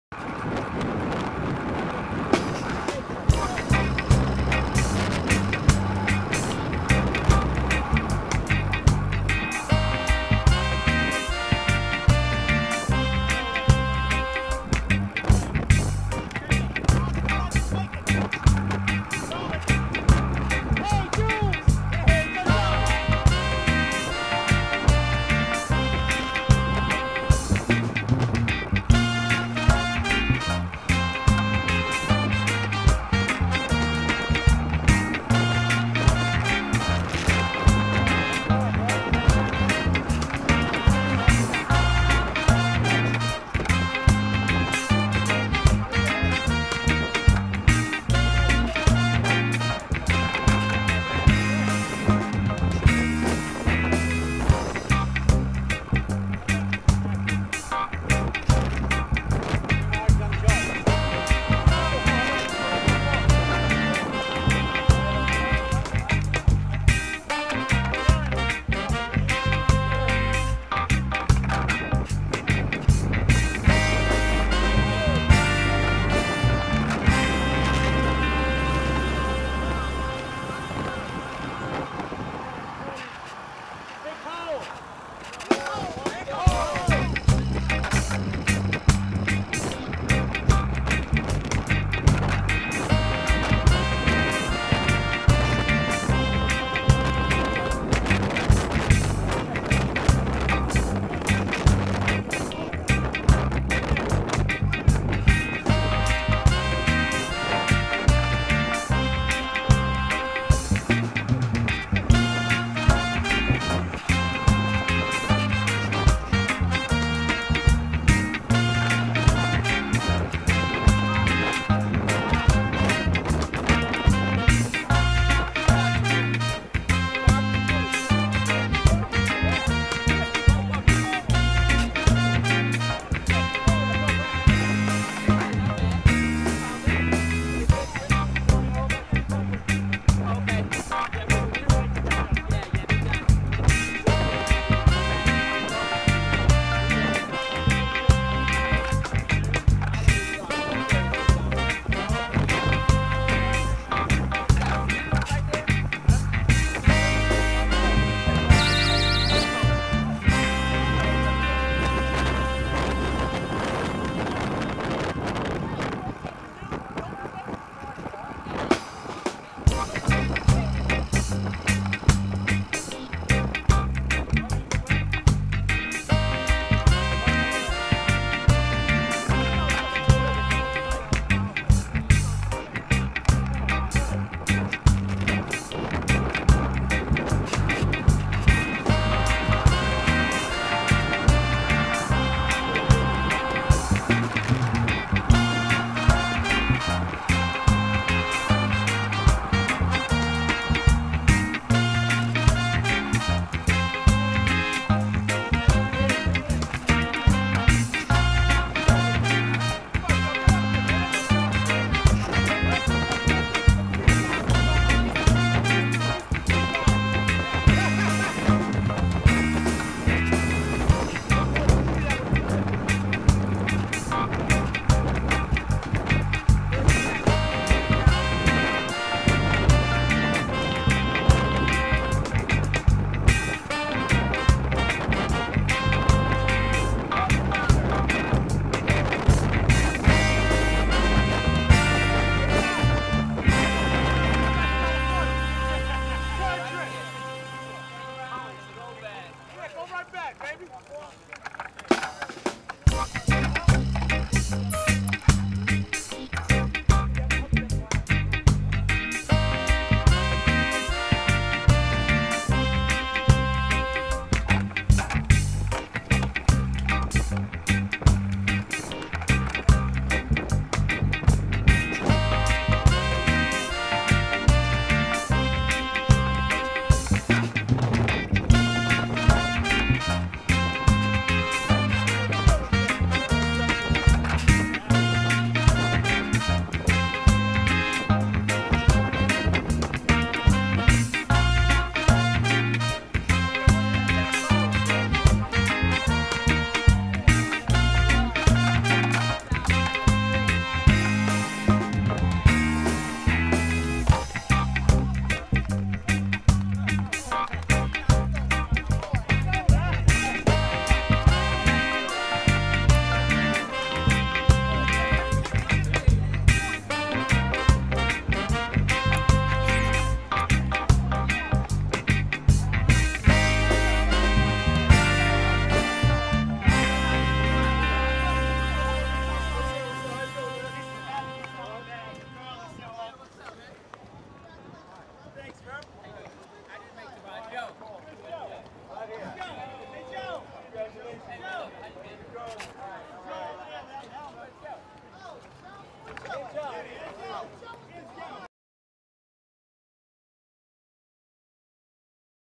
Return to Brick City!
John and Joe return to Brick City... from California! Their cross country road bike trip was from July 17 to August 13 -- what an amazing accomplishment!  They were escorted back into Newark by various riders, including a bunch of Brick City Boneshakers (aka Major Taylor Cycling Club of New Jersey) and members of the Cranford Bike Team.